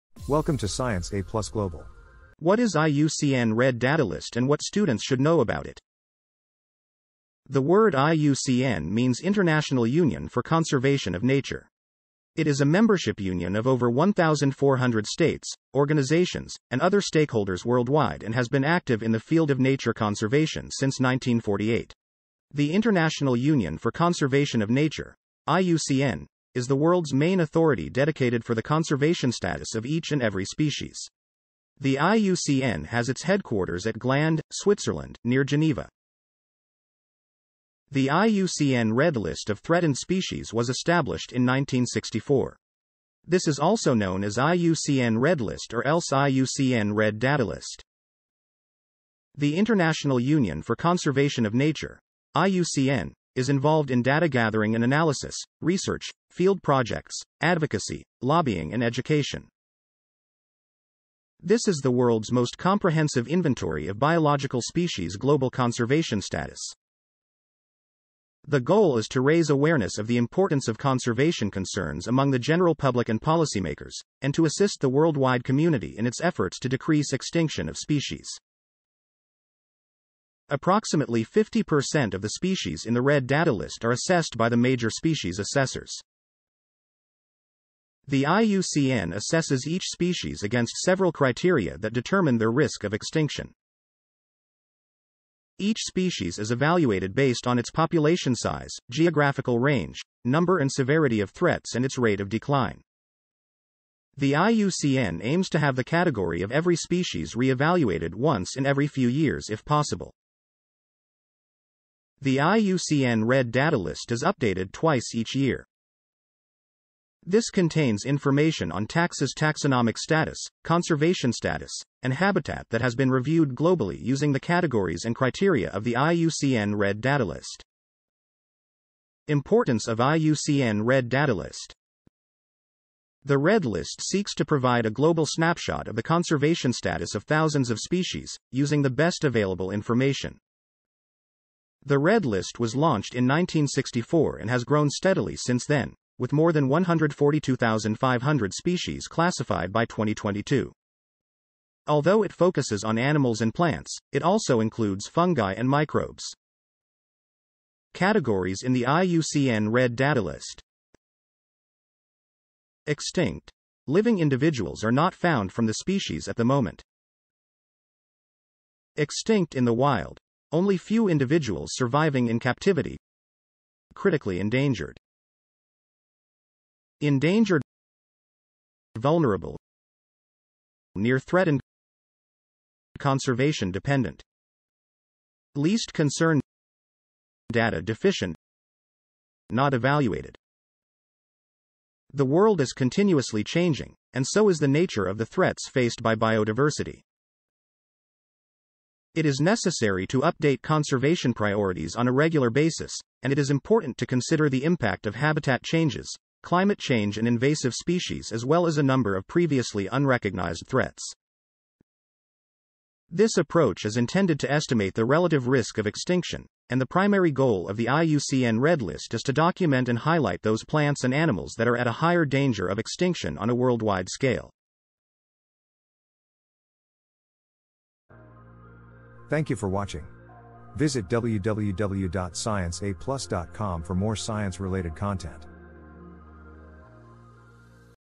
IUCN red data list - What is red data book of species?
What is IUCN?

The word IUCN means International Union for Conservation of Nature. It is a membership union of over 1,400 states, organizations, and other stakeholders worldwide and has been active in the field of nature conservation since 1948.

The International Union for Conservation of Nature (IUCN) is the world’s main authority dedicated for the conservation status of each and every species.

The IUCN has its headquarters at Gland, Switzerland, near Geneva. It was formerly based in Morges, Switzerland.
What is IUCN Red Data List?

The IUCN Red List of Threatened Species was established in 1964. This is also known as IUCN Red List or else IUCN Red Data List.

The International Union for Conservation of Nature (IUCN) is involved in data gathering and analysis, research, field projects, advocacy, lobbying and education.

This is the world’s most comprehensive inventory of biological species’ global conservation status. It employs a set of criteria to assess the danger of extinction for numerous species and subspecies. These requirements apply to all species and all parts of the planet.

The goal is to raise awareness of the importance of conservation concerns among the general public and policymakers, and to assist the worldwide community in its efforts to decrease extinction of species.

Attributions